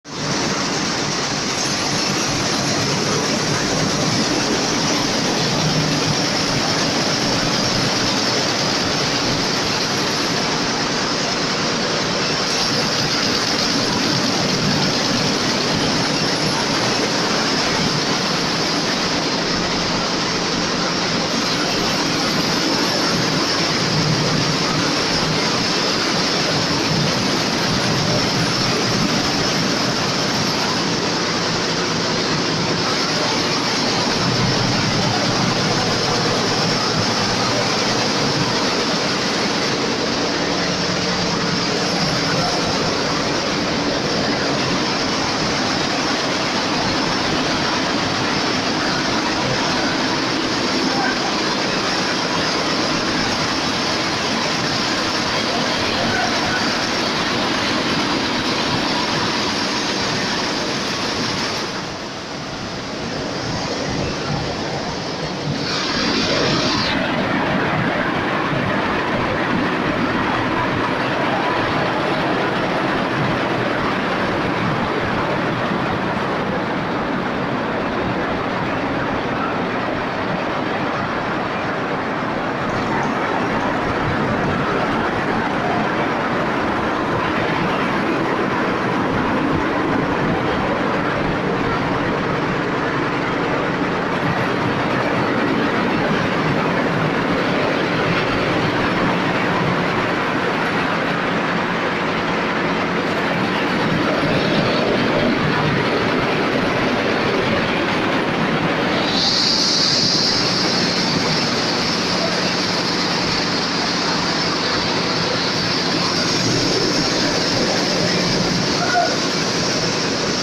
Aaj Hum aap log ko dikhayenge laser machine Mein suit ka kapda Sarika Bada kaise banaya jata hai
Laser machine ke baare mein